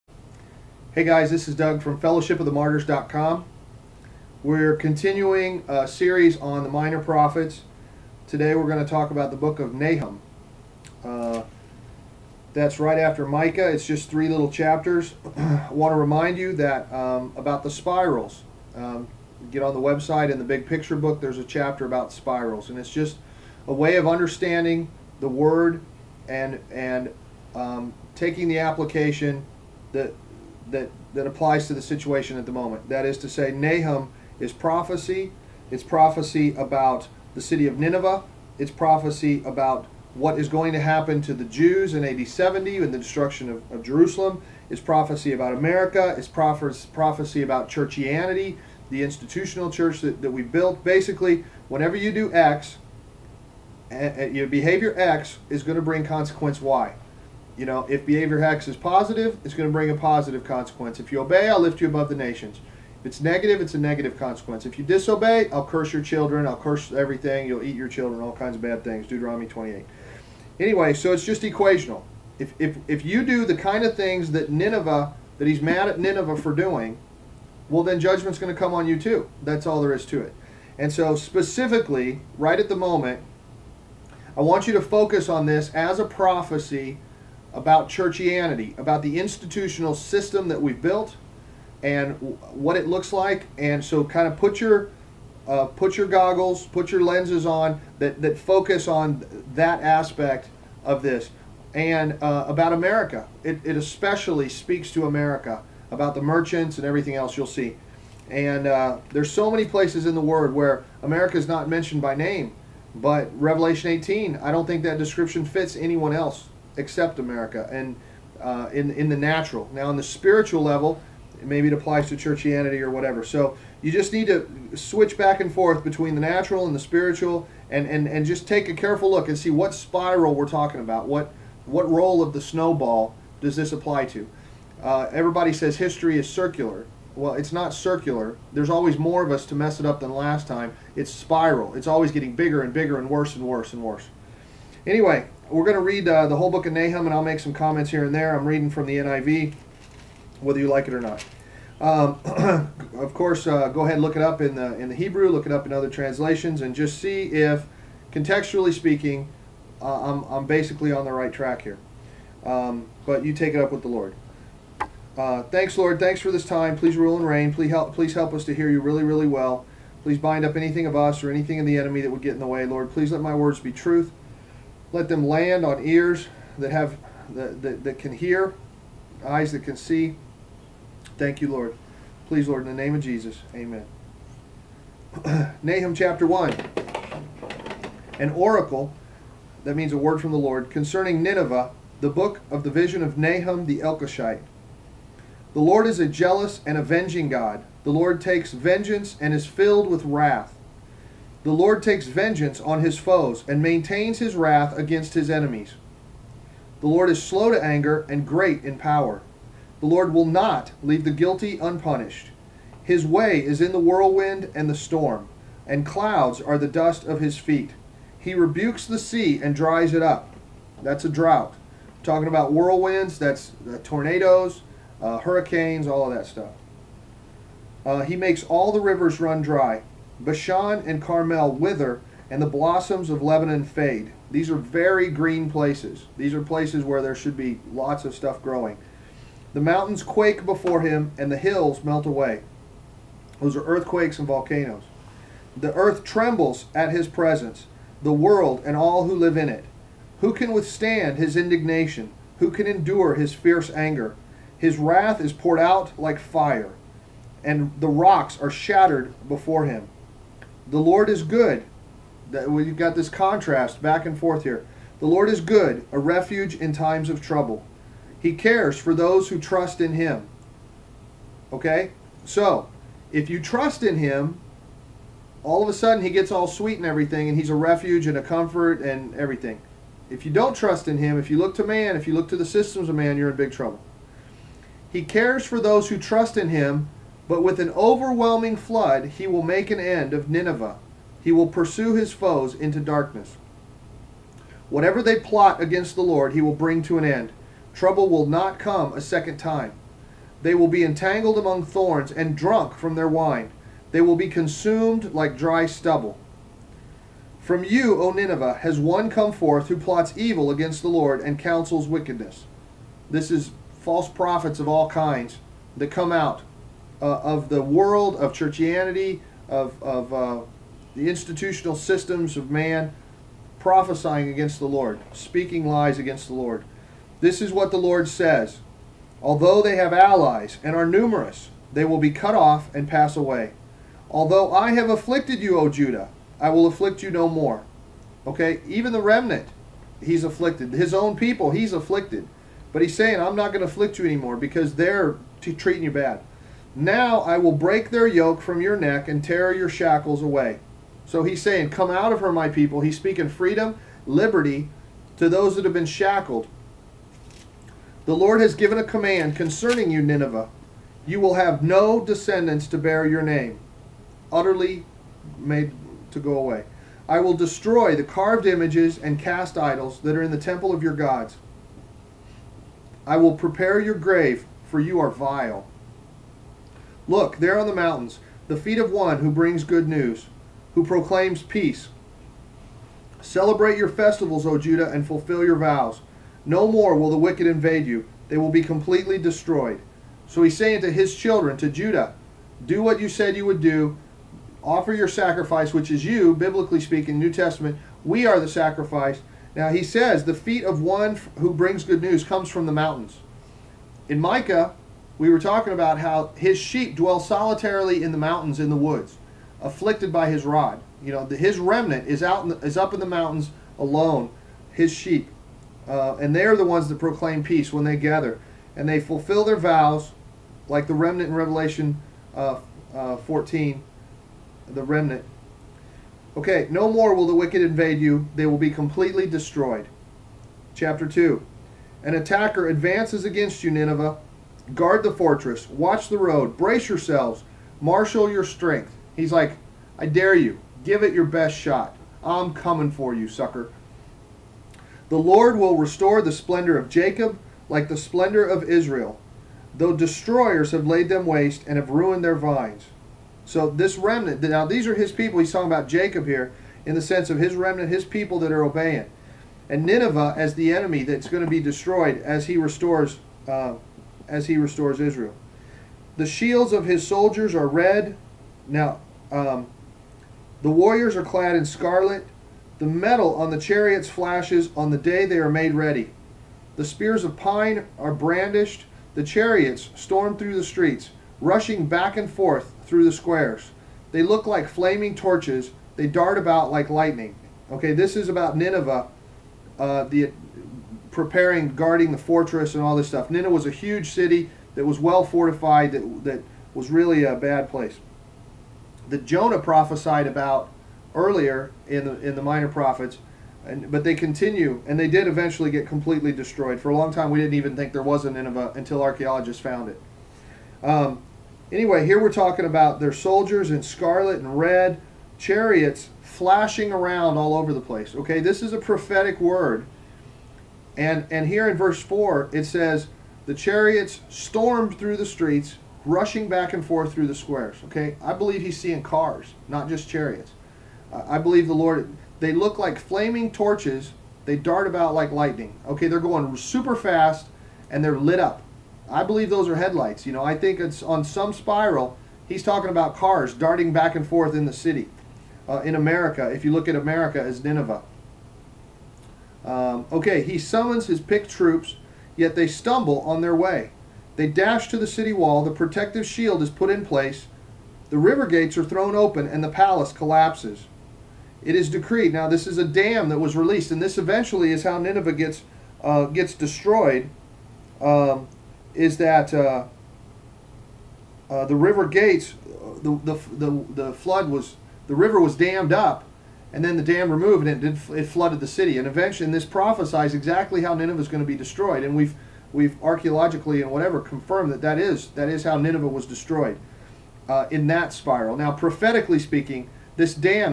Nahum - 1 of 2 - FOTM1
A read-thru and discussion of the book of Nahum and it's application to America and to "churchianity" as we have known it.